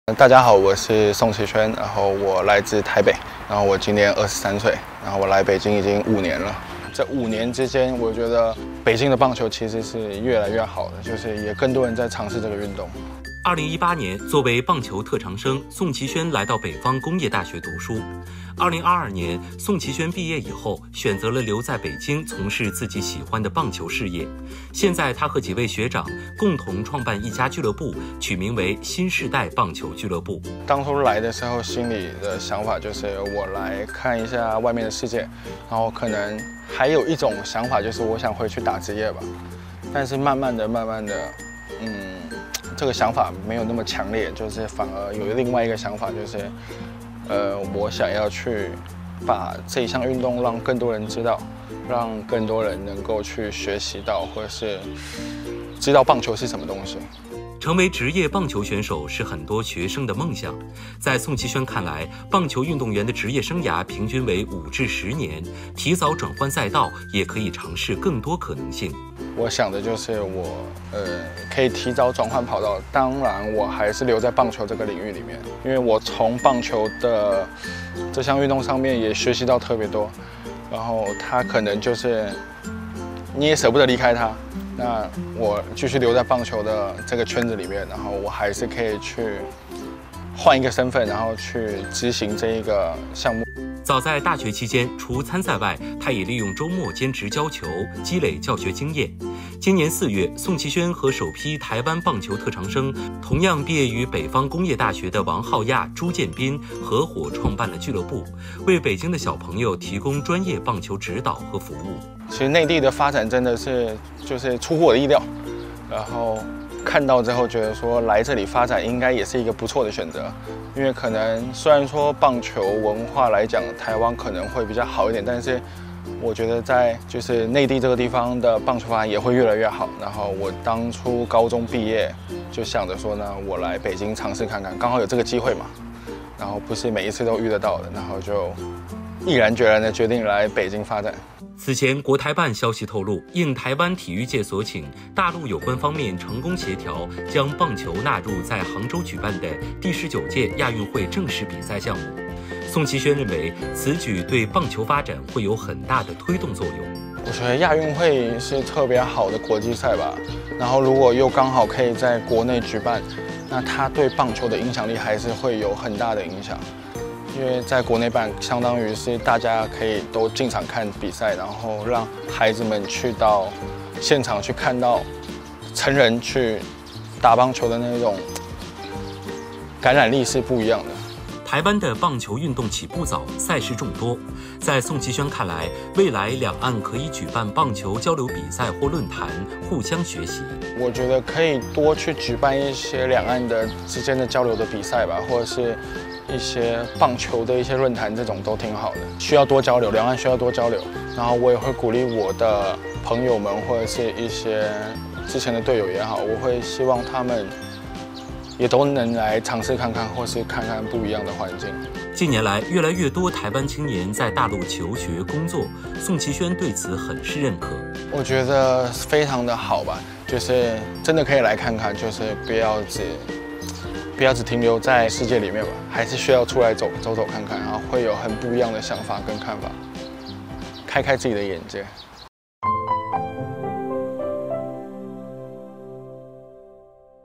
【情牵两岸】台青转换赛道在京创业：从投手到教练 因大陆机会更多
2018年，作为棒球特长生，宋奇轩来到北方工业大学读书，2022年，宋奇轩毕业以后，选择了留在北京从事自己喜欢的棒球事业，现在他和几位学长共同创办一家俱乐部，取名为“新世代棒球俱乐部”。